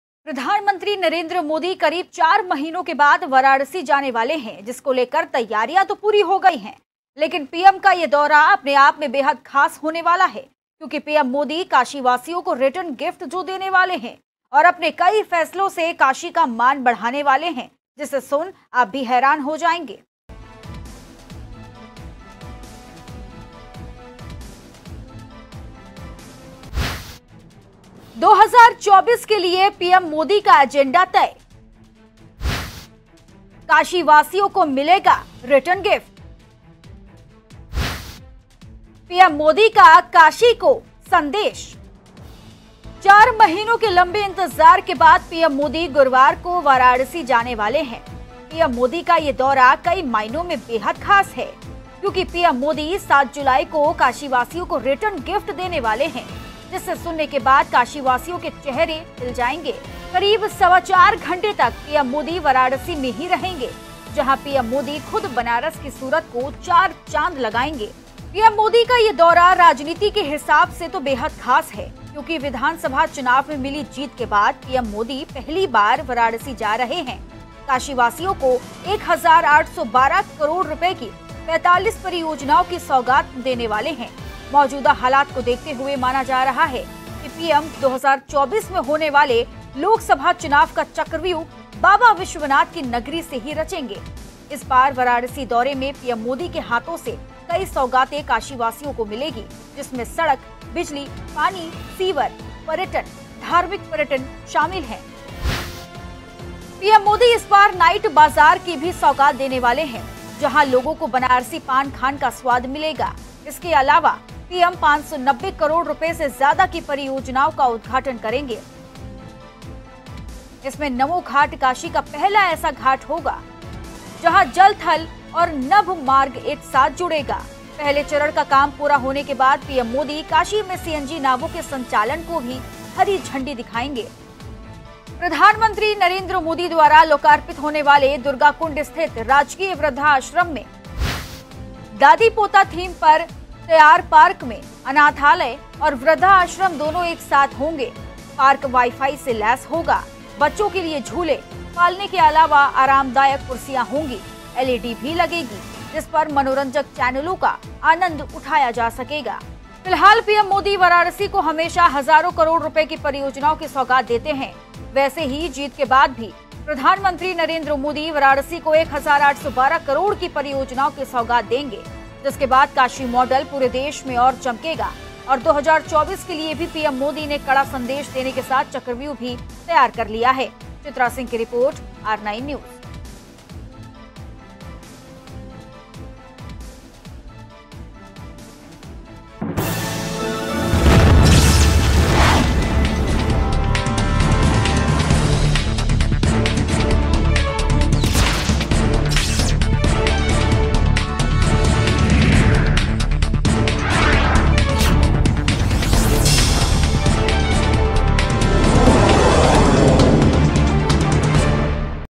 PM MODI बढ़ाएंगे काशी का मान, काशीवासियों को देंगे रिटर्न गिफ्ट.. सुन आप भी हो जाएंगे हैरान...
PM का वाराणसी दौरा खास
4 महीने बाद वाराणसी जाएंगे PM
काशिवासियों को रिटर्न गिफ्ट देंगे PM
PM मोदी बढ़ाएंगे काशी का मान